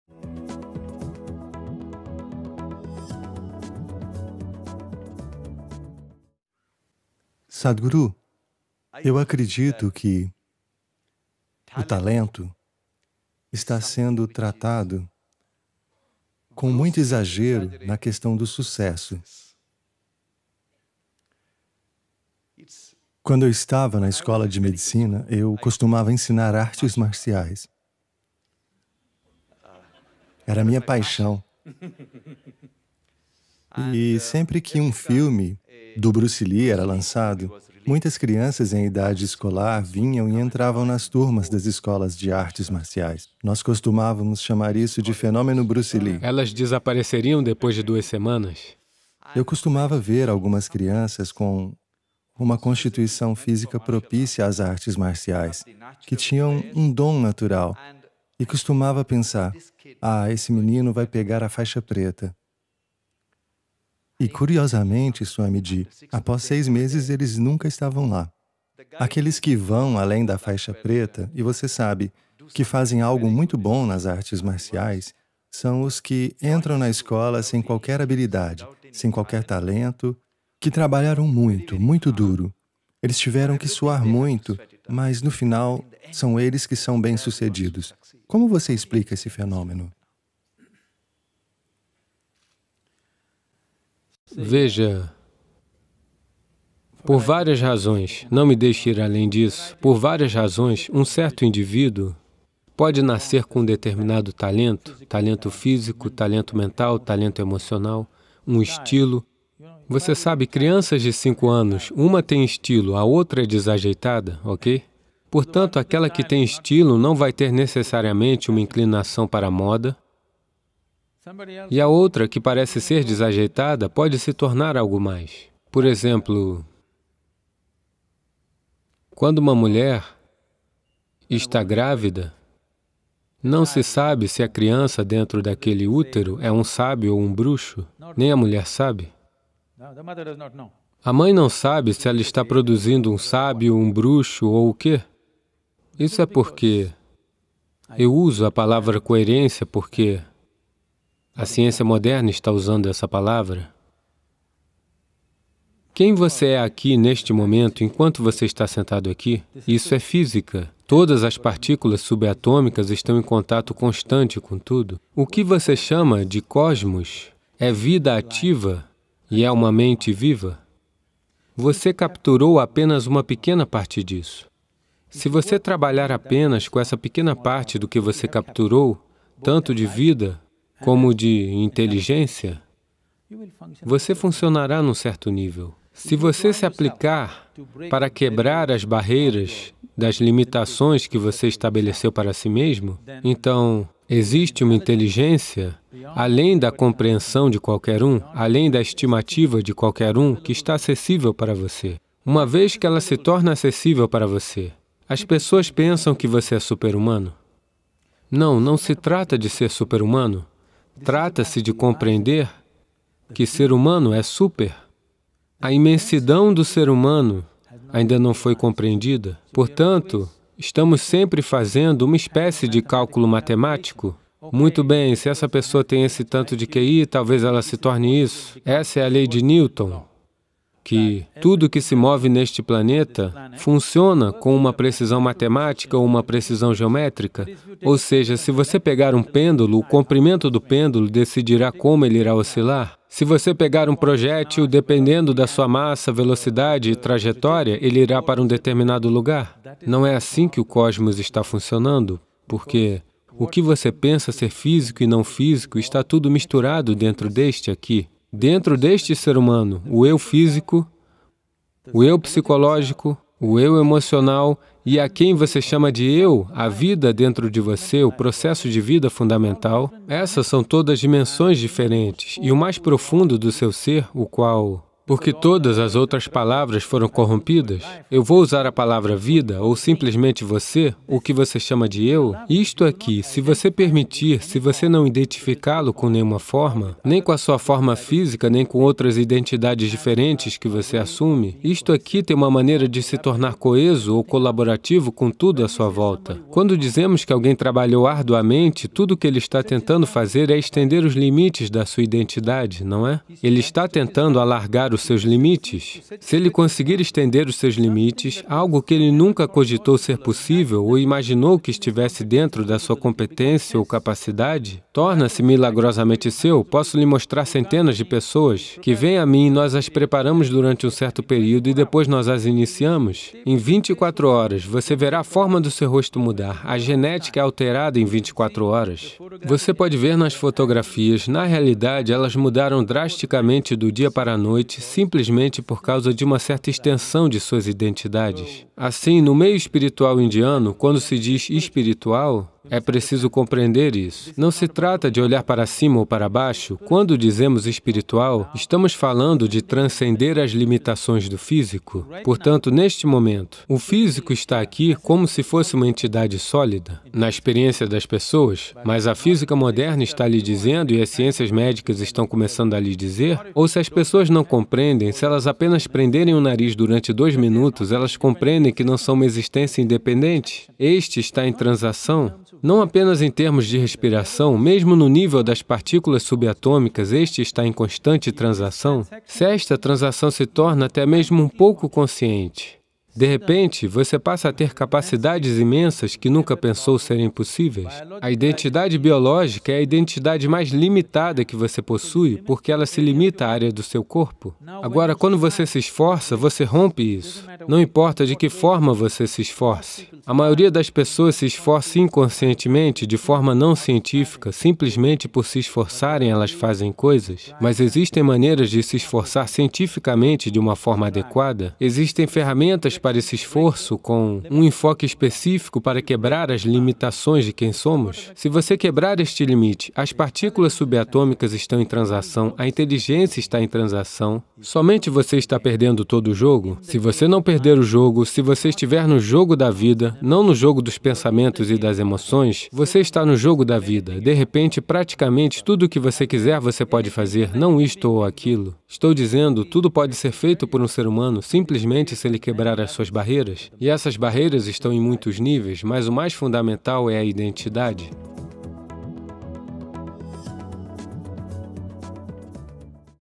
Desbloqueando o Potencial Humano | Sadhguru Português
O renomado cirurgião cardíaco, Dr. Devi Shetty, pergunta a Sadhguru sobre o papel que o talento e o trabalho árduo desempenham no aumento de nossas capacidades. Sadhguru fala sobre como os seres humanos são imensamente capazes se romperem as muitas barreiras que impõem a si mesmos.